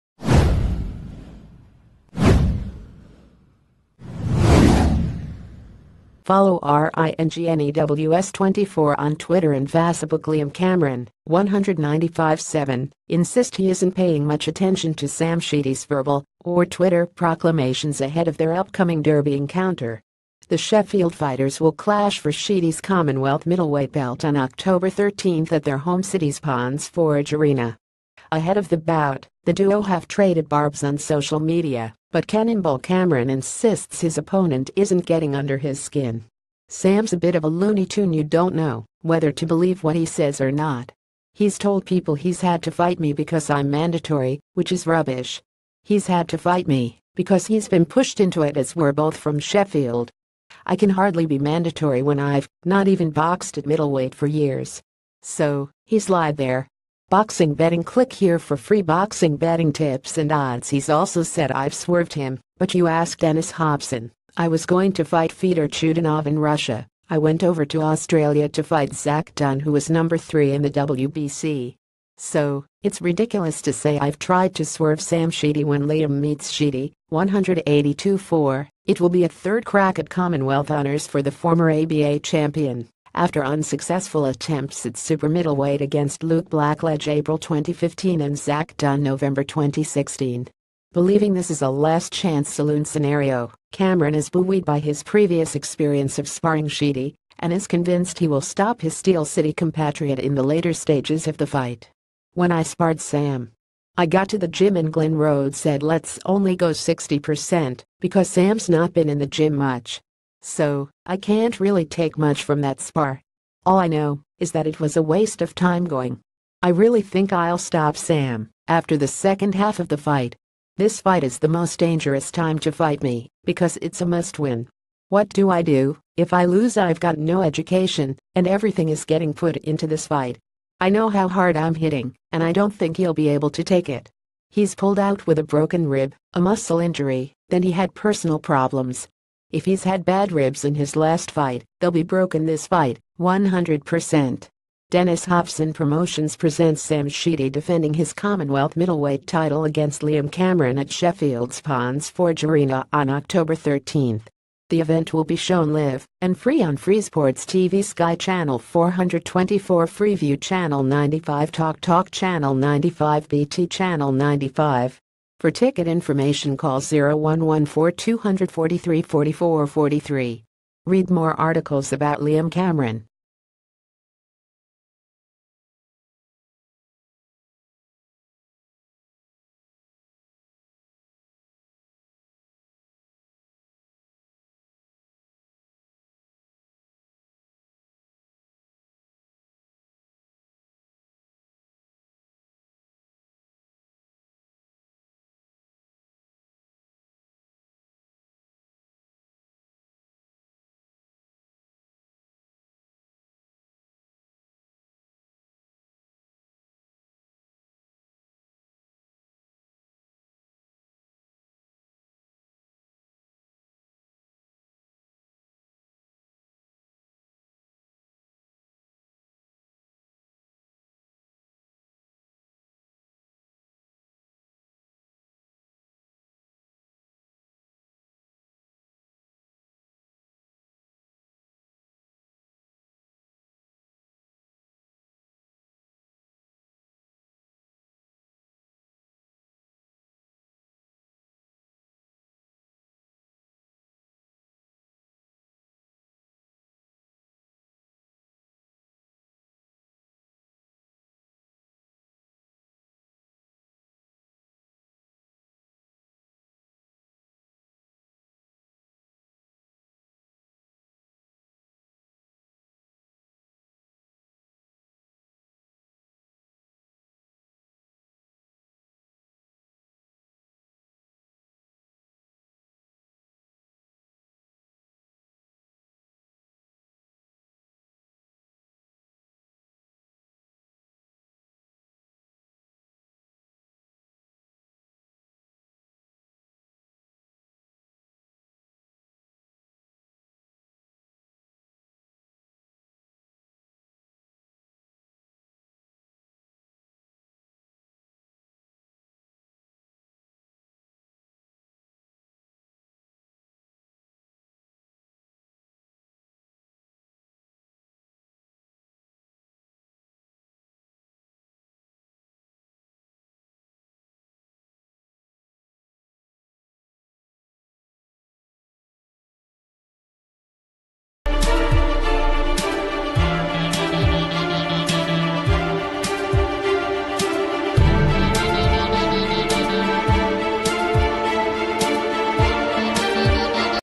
Breaking News | Cameron: “i’ll stop sheedy to win commonwealth”
Cameron: “i’ll stop sheedy to win commonwealth”

FOLLOW RINGNEWS24 ON TWITTER AND FACEBOOKLiam Cameron, 195 7, insist he isn’t paying much attention ...